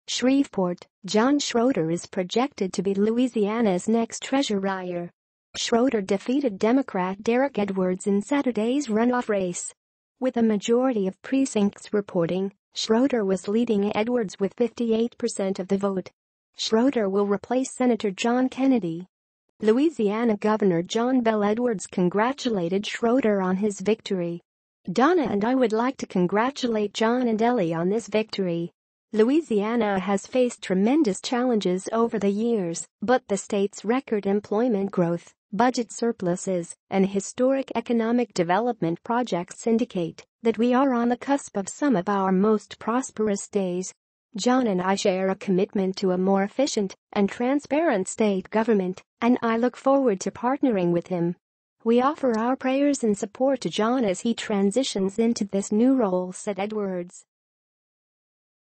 John Schroder wins Louisiana Treasurer race
SHREVEPORT - John Schroder is projected to be Louisiana's next Treasurer.

Schroder defeated Democrat Derrick Edwards in Saturday's runoff race.

With a majority of precincts reporting, Schroder was leading Edwards with 58 percent of the vote.

Schroder will replace Sen. John Kennedy.

Louisiana Governor John Bel Edwards congratulated Schroder on his victory.

“Donna and I would like to congratulate John and Ellie on this victory. Louisiana has faced tremendous challenges over the years, but the state’s record employment growth, budget surpluses, and historic economic development projects indicate that we are on the cusp of some of our most prosperous days. John and I share a commitment to a more efficient and transparent state government, and I look forward to partnering with him. We offer our prayers and support to John as he transitions into this new role" said Edwards.